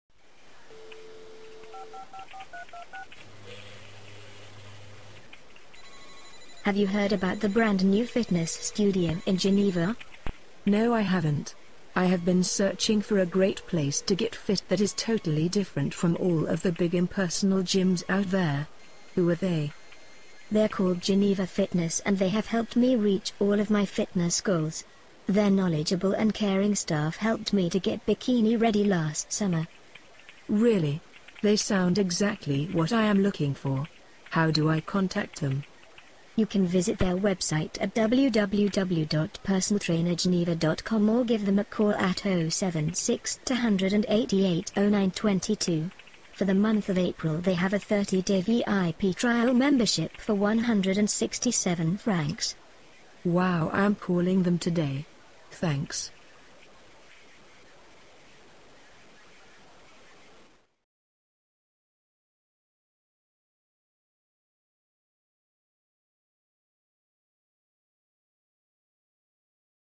New Fitness Studio In Geneva, Switzerland!
New to Geneva!  Geneva Fitness Coaching Studio.  Let us be your answer to all of your health and fitness needs.